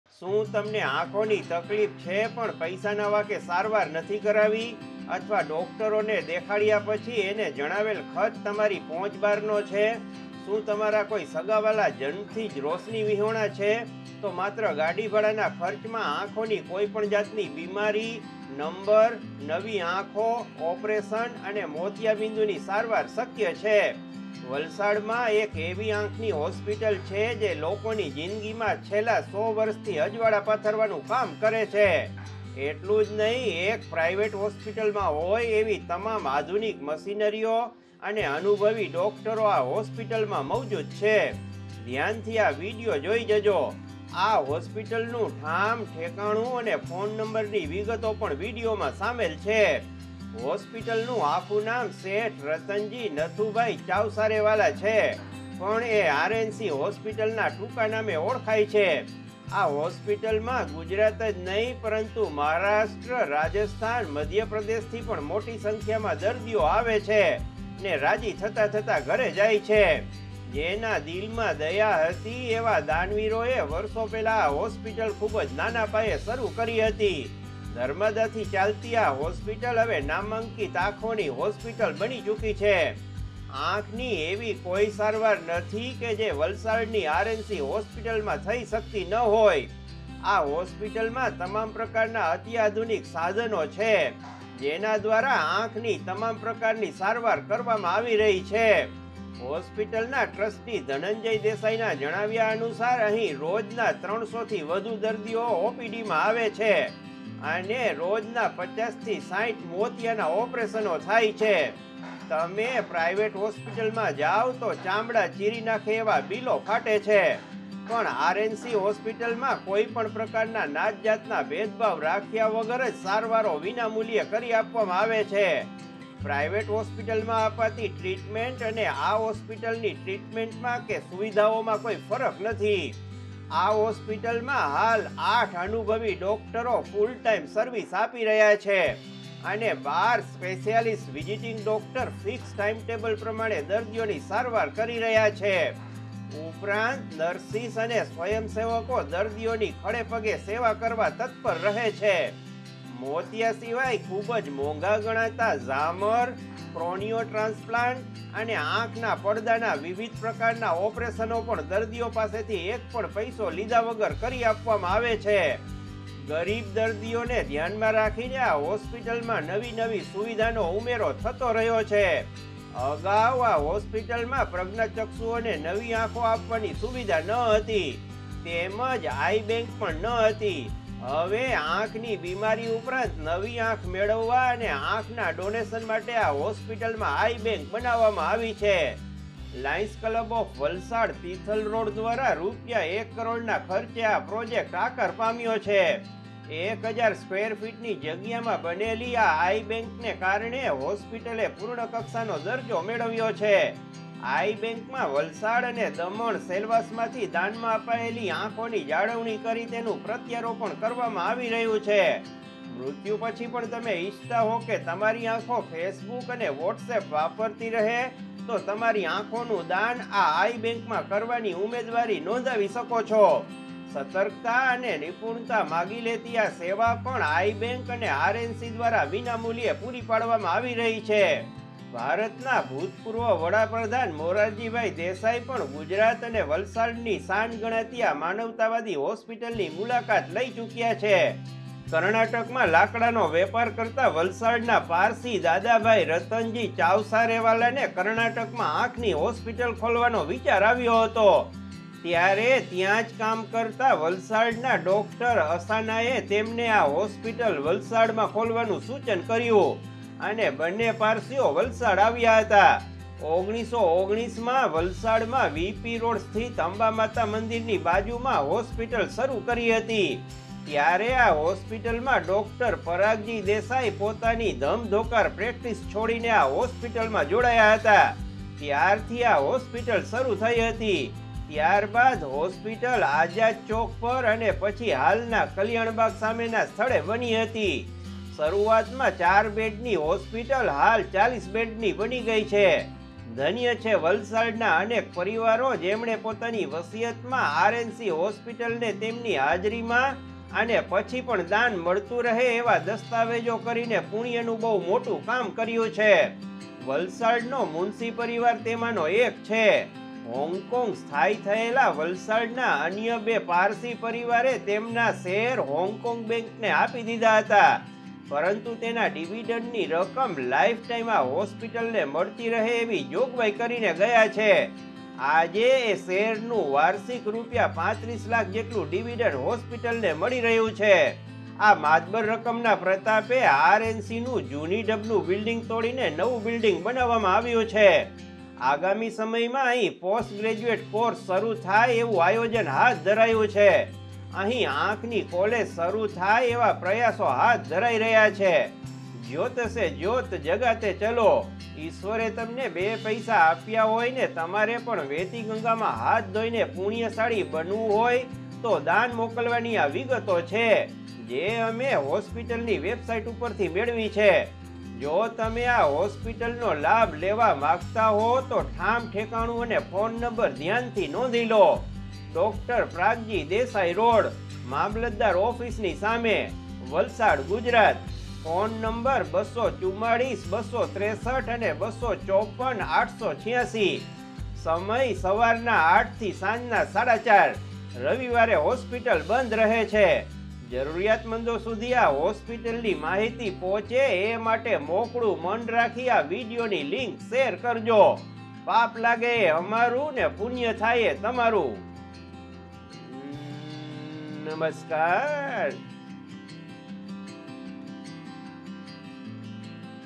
અહીં લાખોના ઓપરેશનો થાય છે સાવ મફત । RNC Free Eye Hospital | Tithal | Valsad
The RNC Free Eye Hospital is situated in Tithal, Valsad, Gujarat. It was founded in 1920 by Shri Khanbahadur Sheth Dadabhai Ratanji Balsara (Chavsarewala), and provides free of cost Ophthalmology Treatments since beginning. It is an internationally-renowned eye institution that specializes in the diagnosis and management of complex medical and surgical eye disease; and serves as a preeminent provider of routine eye care and refractive, optical, and eye trauma services.